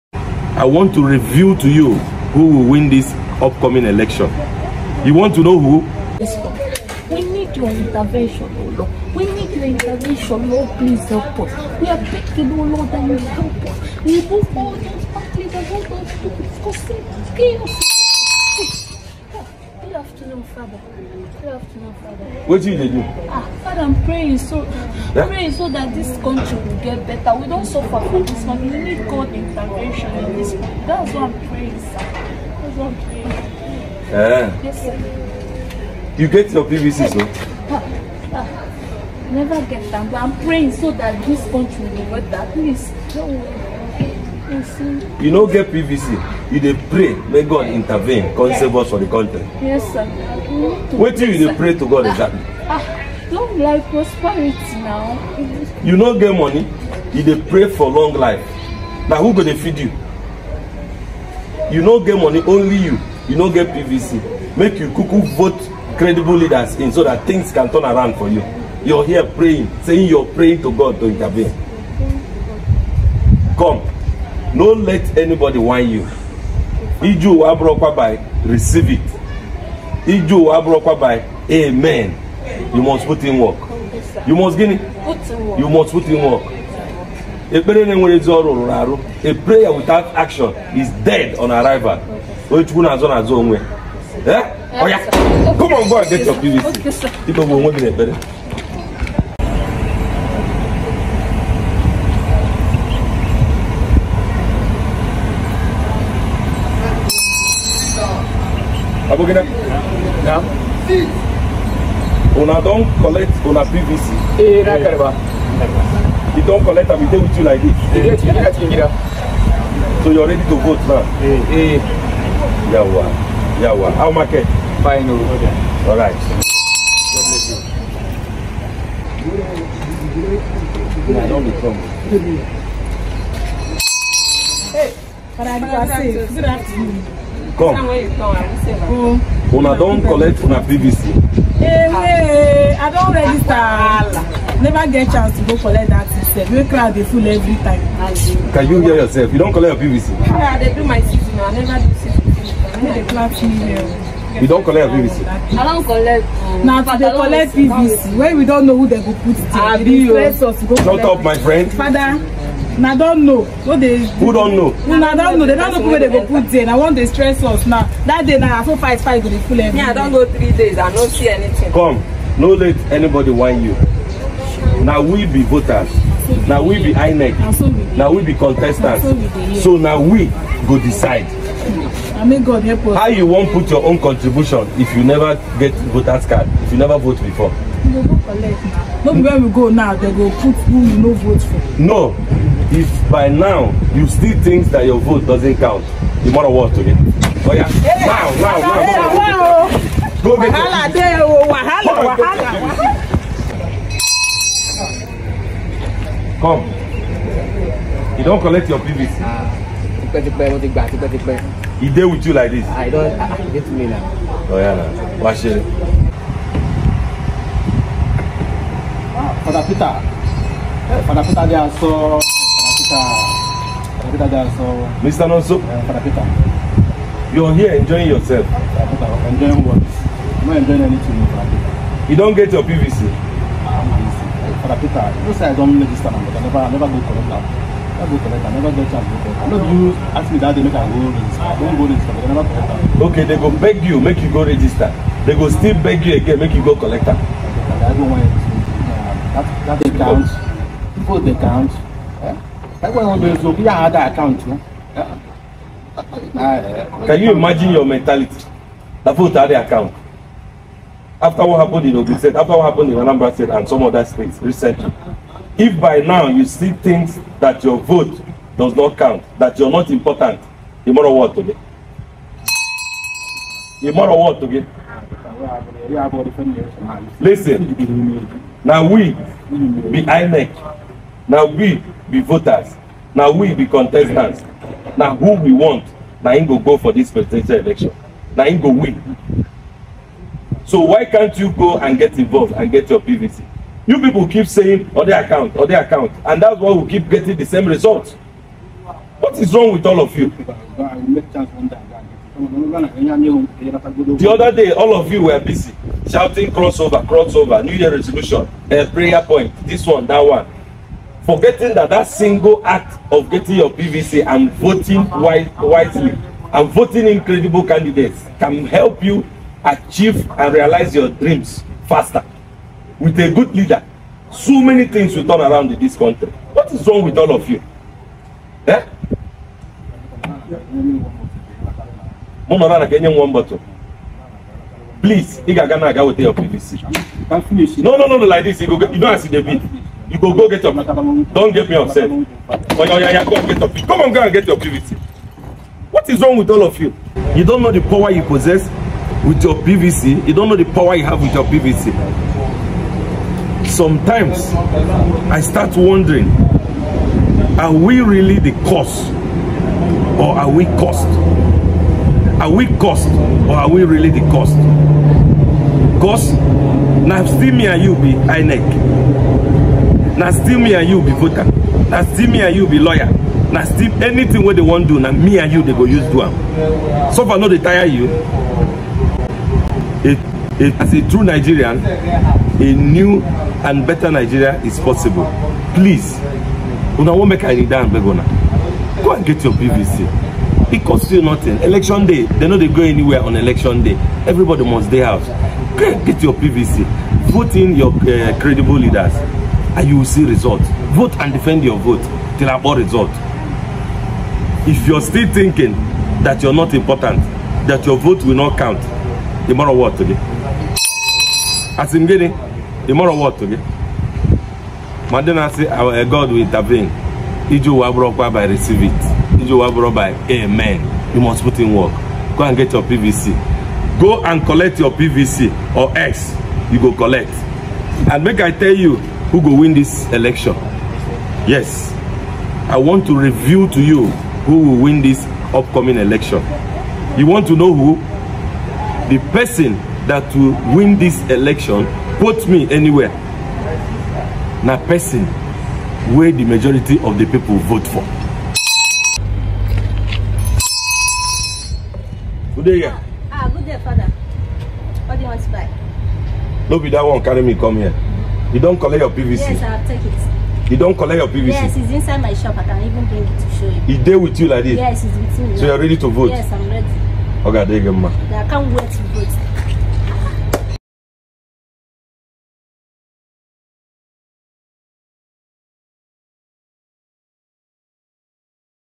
CATH0L1C PRIEST BOLDLY NAME WINNER FOR D NEXT PRES1DENT1AL ELECT10N AT OPERATION COLLECT UR PVC
Wow Fada Peter boldly announces the winner for the upcoming election as he declares operation collect your pvc ,this is the first man of God to openly reveal this ,plz carefully WATCH, LEARN AND SHARE THIS ,LIKE, COMMENT and make sure you SUBSCRIBE to this channel for steady update and Vawulence